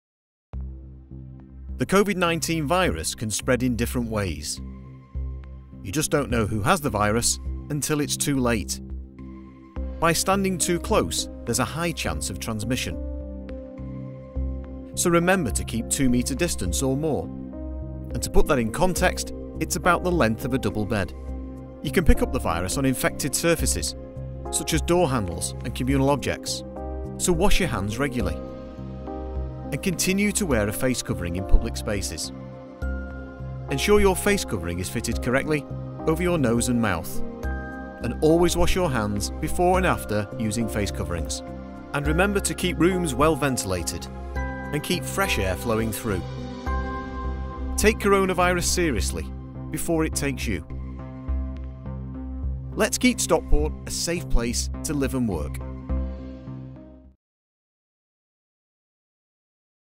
How the Covid-19 virus spreads (2021)
This video explains how coronavirus spreads and how easily it spreads

Hands, face, space and ensure rooms are well ventilated.